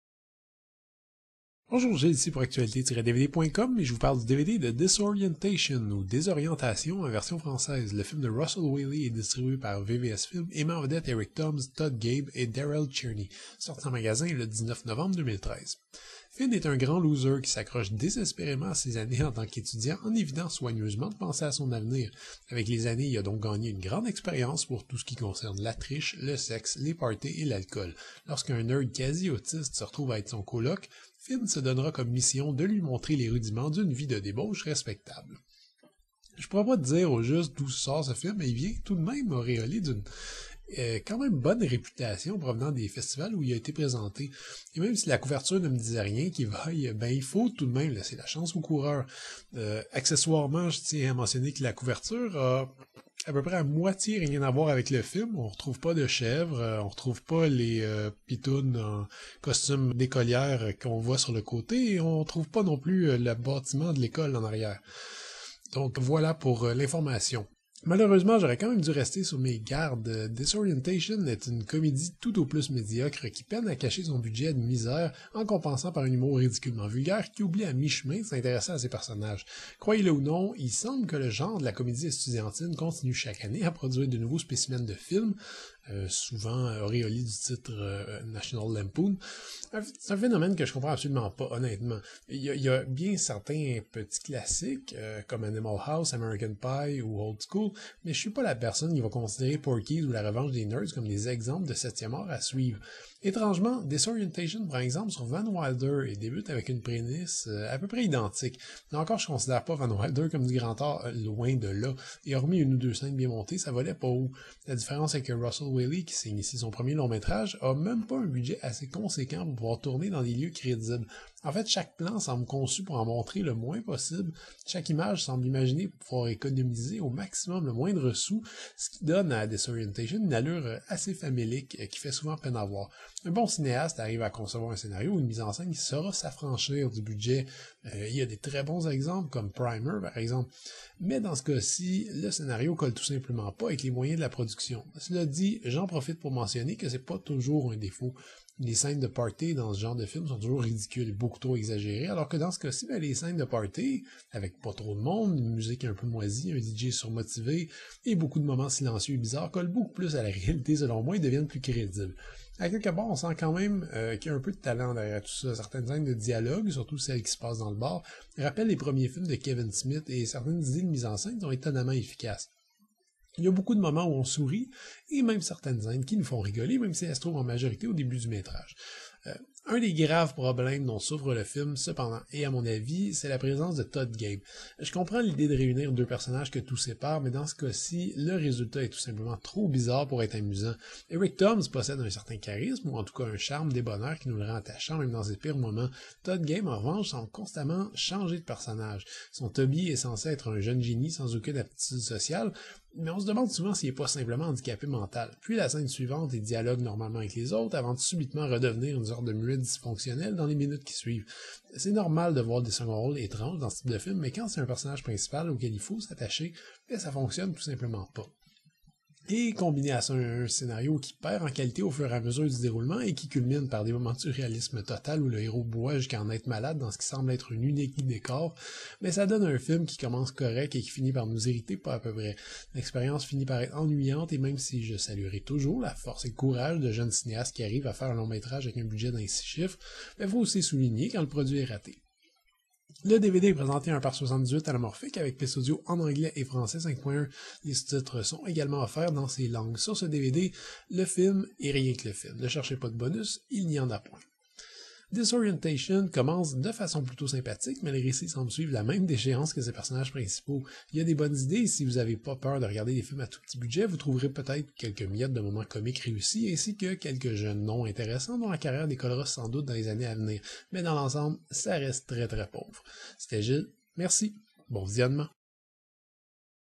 Critique DVD Disorientation
Critique du DVD Disorientation par Gilles pour Le film de Russell Whaley est distribué par VVS Films et met en vedette Eric Toms, Todd Gaebe et Darrel Cherney. Sortie en magasins le 19 novembre 2013.